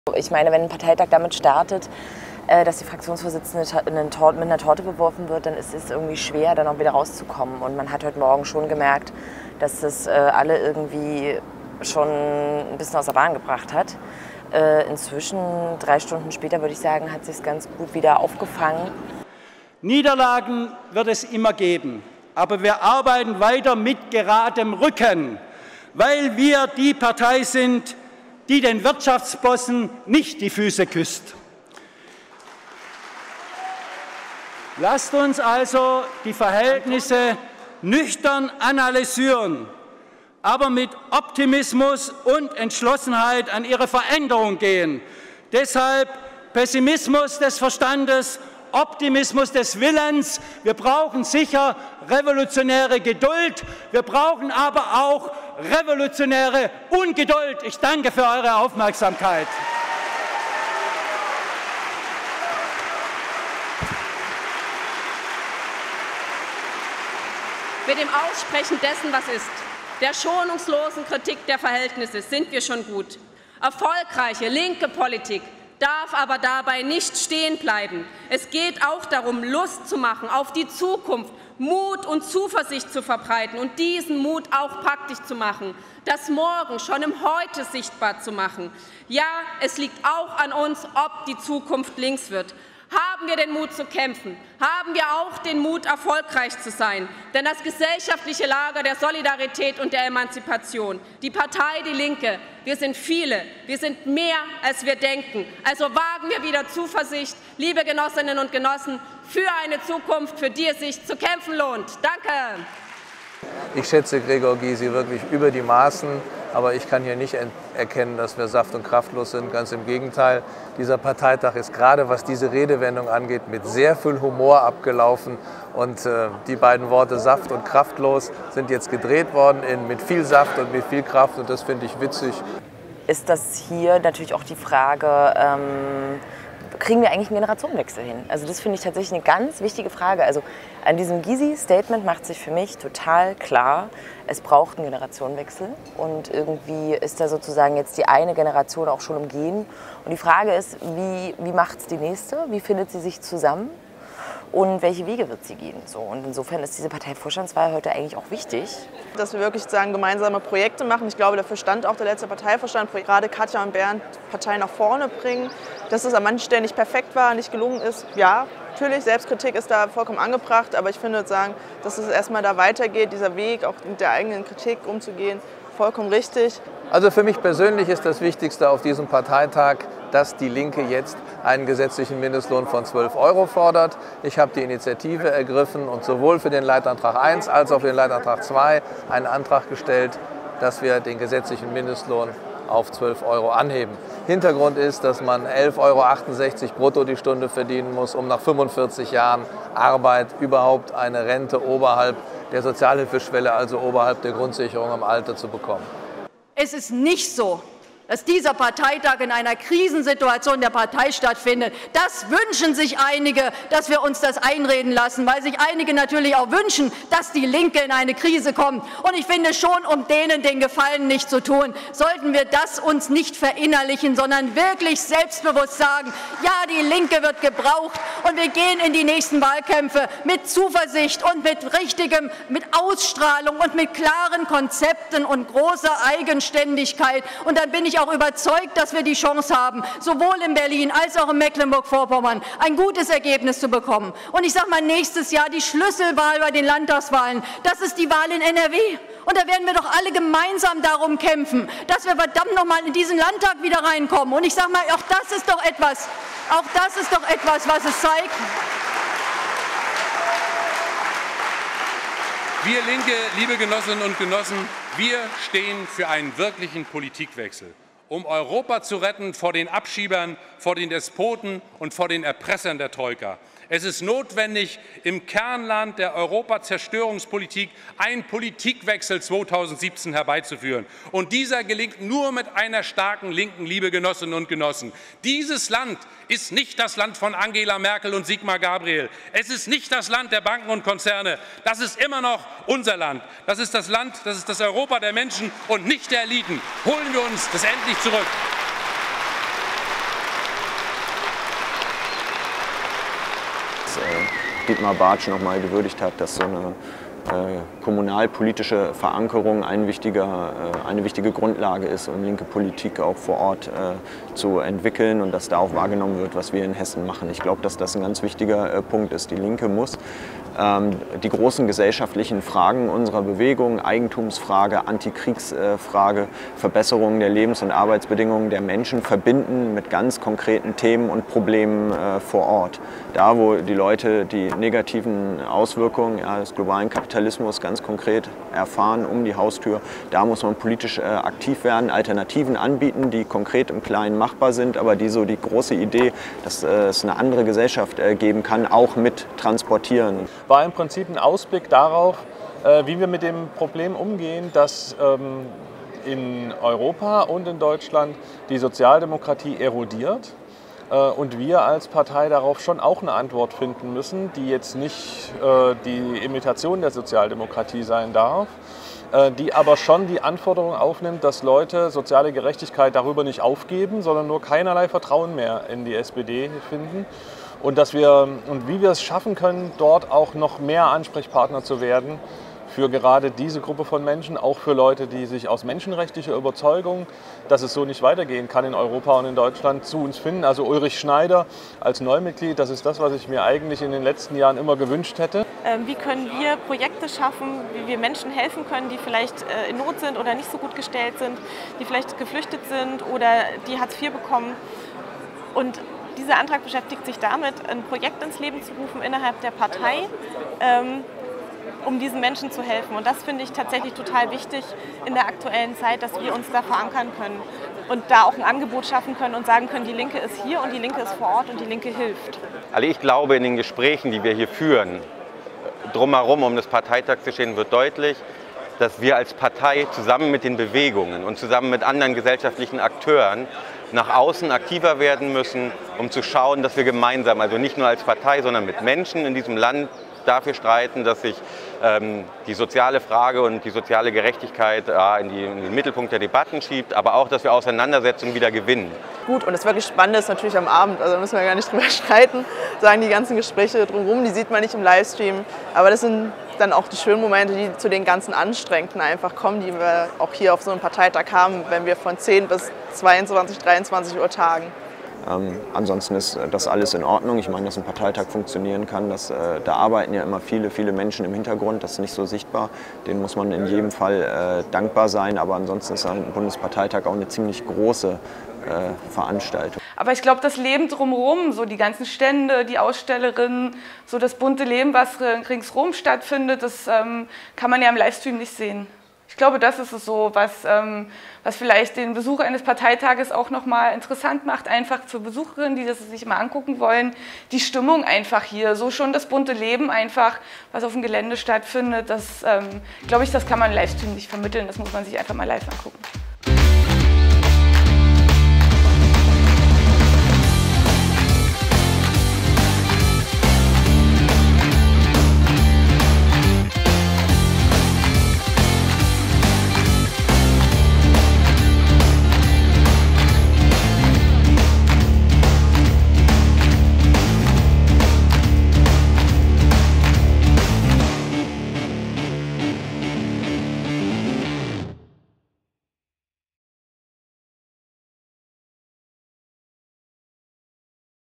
Parteitag in Magdeburg
580 Delegierte, hunderte Teilnehmerinnen und Teilnehmer mit beratender Stimme und Gäste, Beschlüsse zur sozialen Offensive und zur Solidarität mit Geflüchteten, zu Frieden und zum gerechten Welthandel sowie zum Kampf für Demokratie und gegen den Rechtsruck in der Gesellschaft. Die Vorsitzenden Katja Kipping und Bernd Riexinger und der Bundesgeschäftsführer Matthias Höhn wurden bestätigt, ein neuer Vorstand gewählt. Am Wochenende haben wir gezeigt: Wir sind die Partei, die für den sozialen Aufbruch in der Gesellschaft steht und dafür mobilisiert. Die Partei, die keine Ruhe gibt, bevor es nicht besser wird. Sozial gerecht. Für alle. Besser. Unser Bericht vom Parteitag: